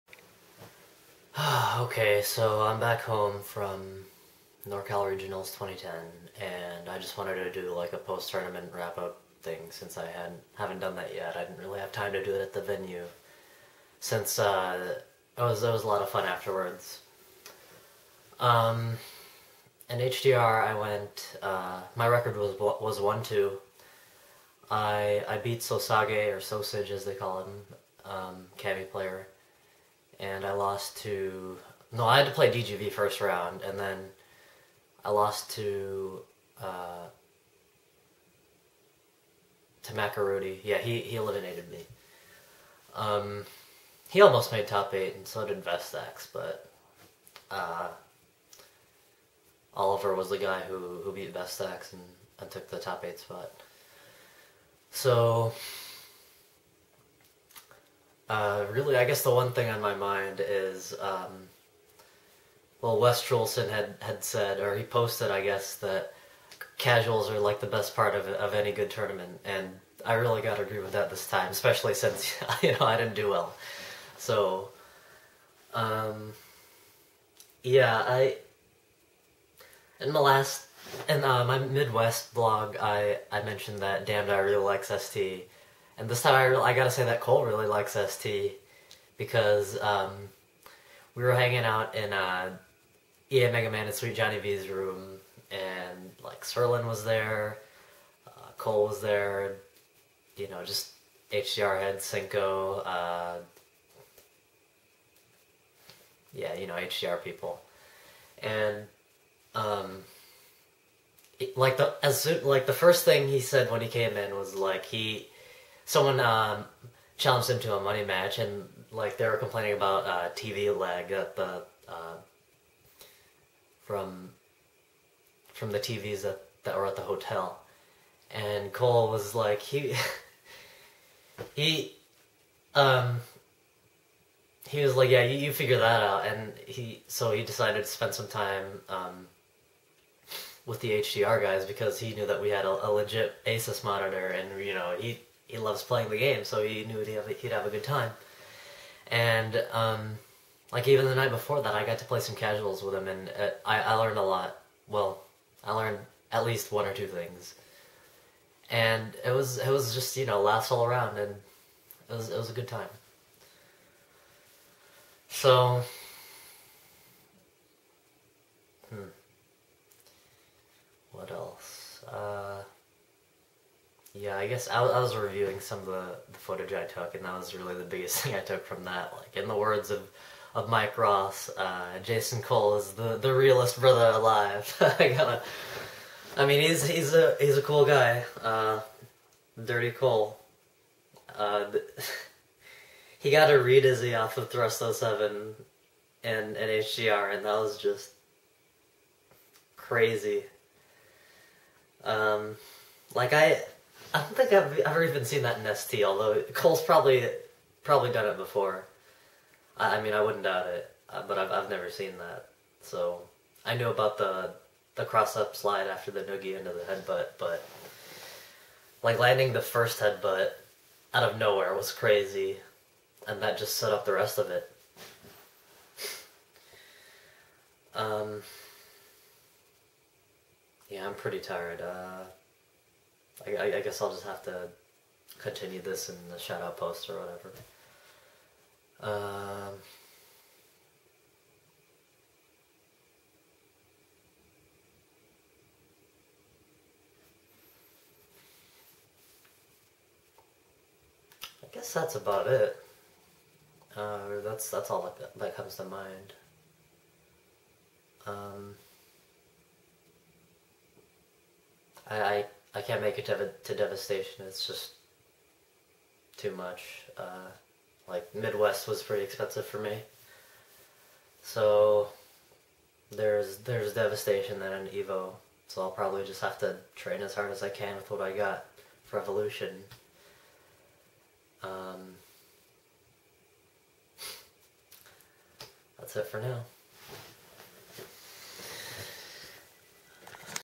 Super Norcal Regionals 2010 Wrap-up
Northwest scene is lucky to have Cole. We did a little team tournament before we left NCR. Cole and Sirlin were team captains who did a draft (they played a 2/3 set for first pick).